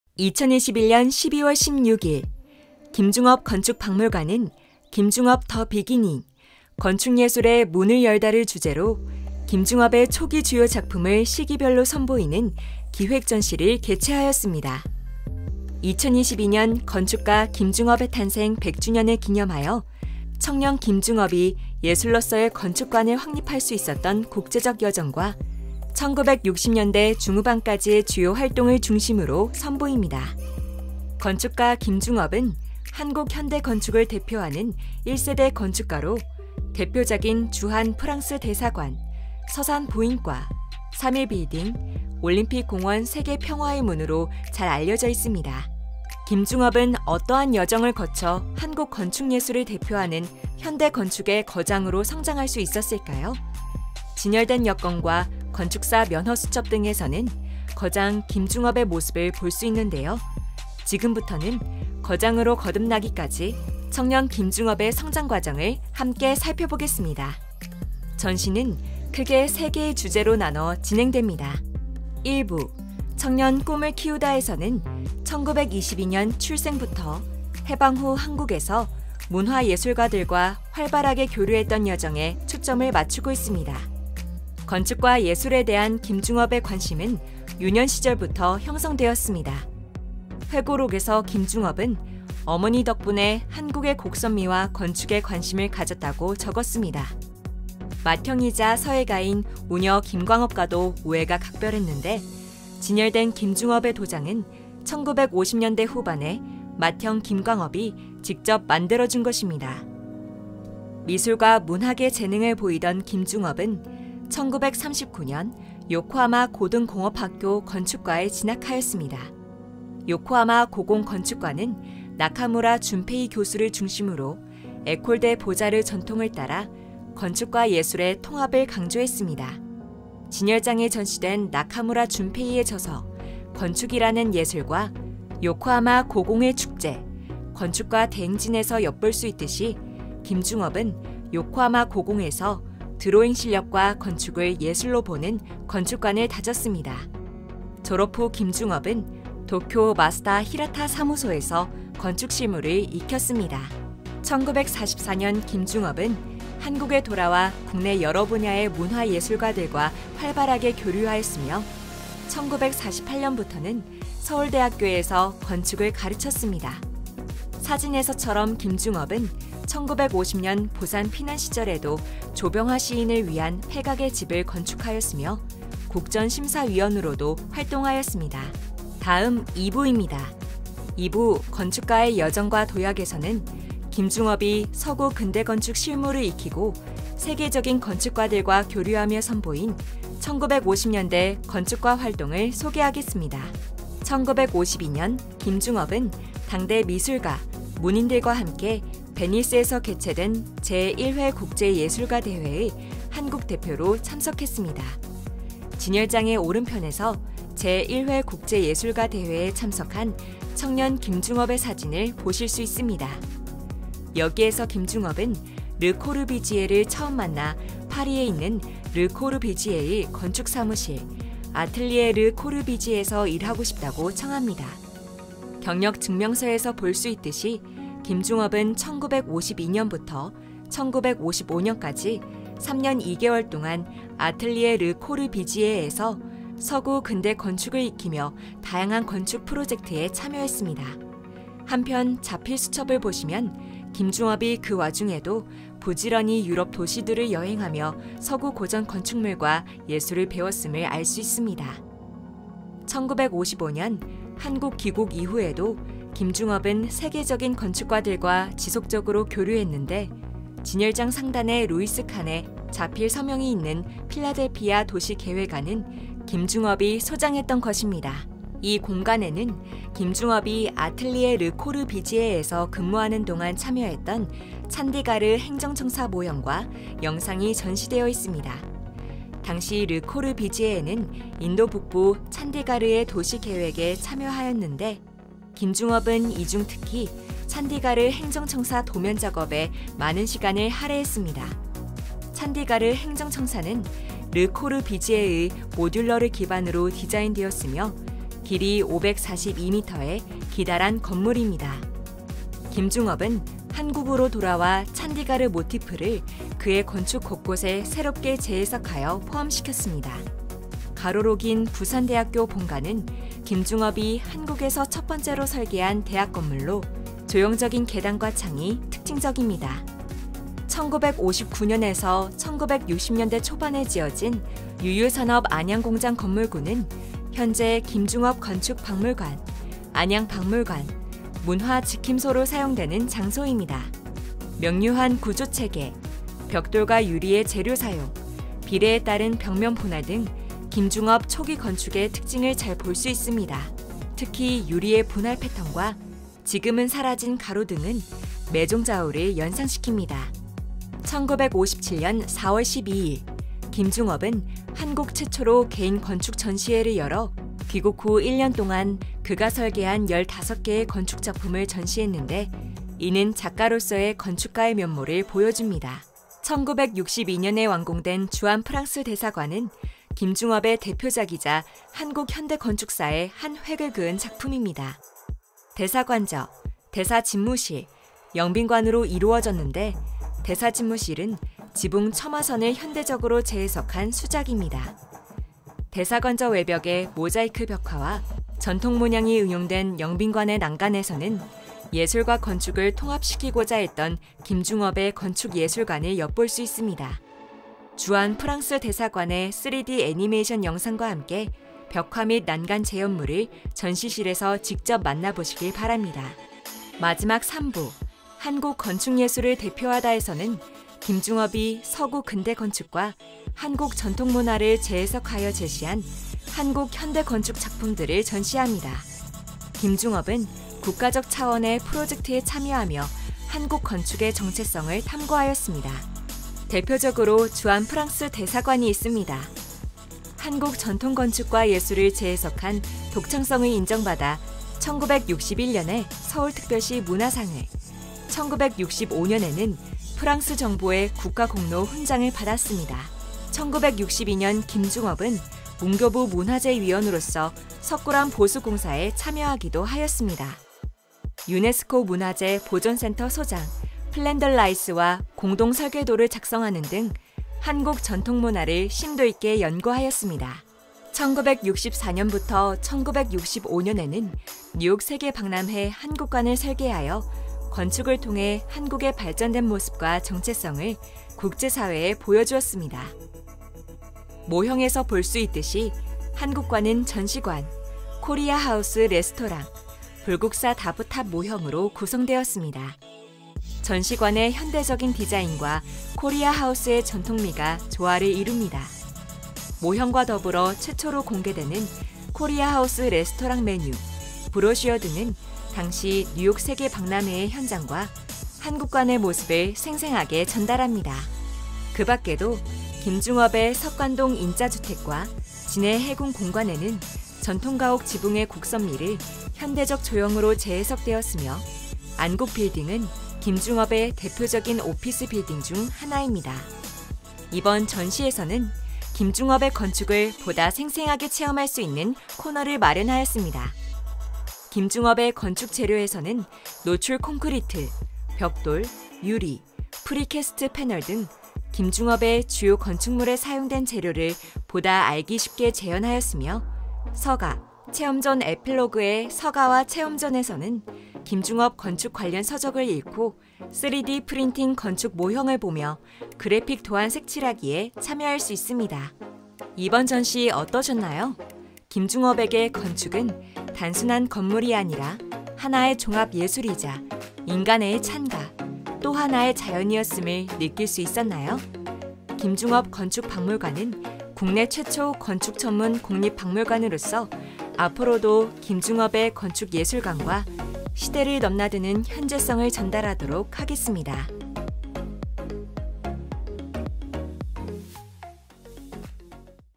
[김중업 더 비기닝][10분 해설투어] 기획전시 10분 온라인 전시해설
2021 김중업건축박물관 기획전시
[ 김중업, 더 비기닝: 건축예술의 문을 열다 ]
온라인 전시해설 영상입니다.
기간: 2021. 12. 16. ~ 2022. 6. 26.
장소: 김중업건축박물관 1층 전시실